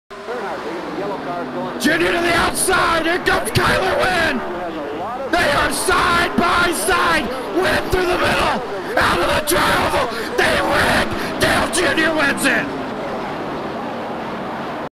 dale allison comparison
just a side by side on how identical these blowovers are, they happened in the same spot almost and the same direction and such